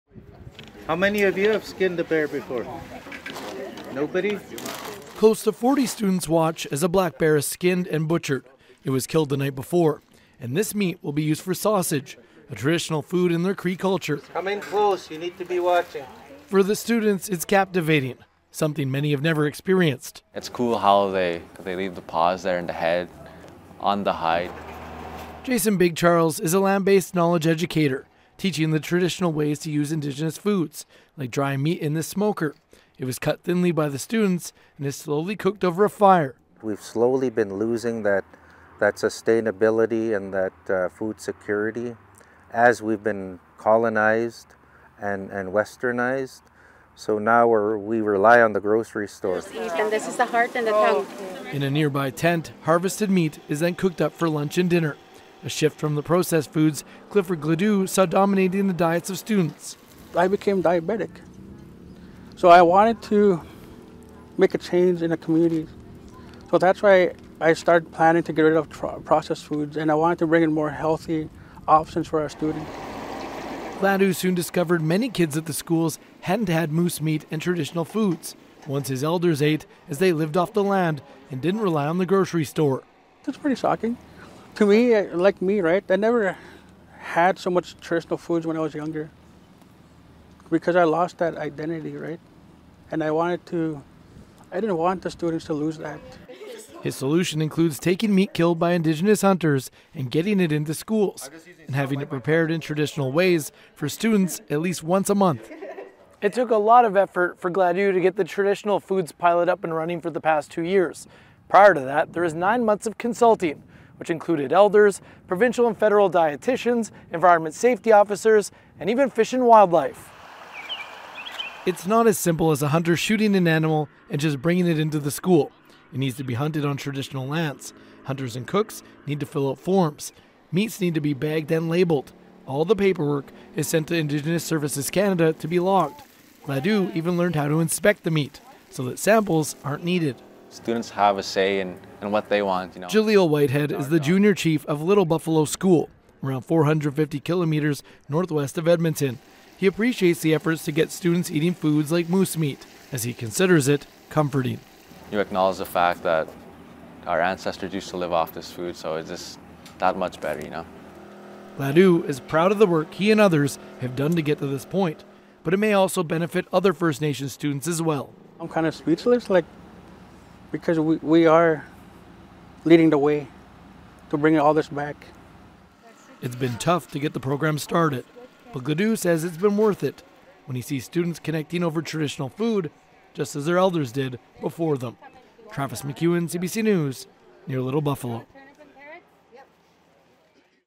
Food program connects First Nations students with traditions of elders
White Fish Lake Nation member Clifford Gladue is working to connect students in northern Alberta with traditional food like moose meat. The pilot project serves hunted meat as part of a food program in six Cree schools.